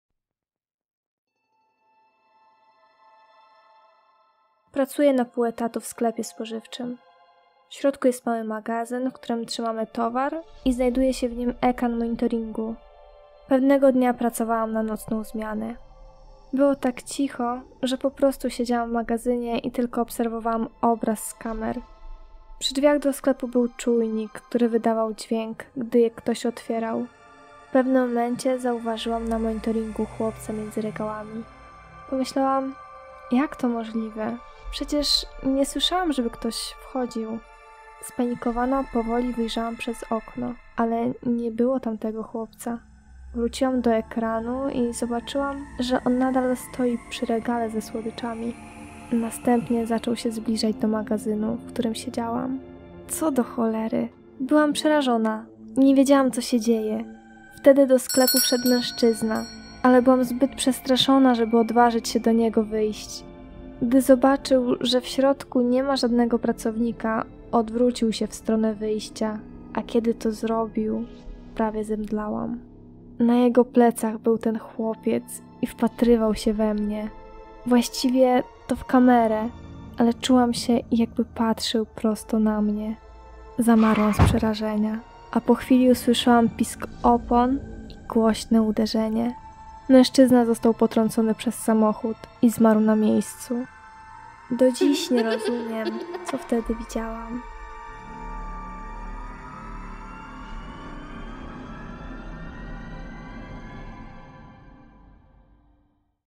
Zobaczyłam na monitoringu przerażającego chłopca
Historia oparta na prawdziwych przeżyciach widza.

Muzyka: Dan Bodan
Animacja: Lubi chae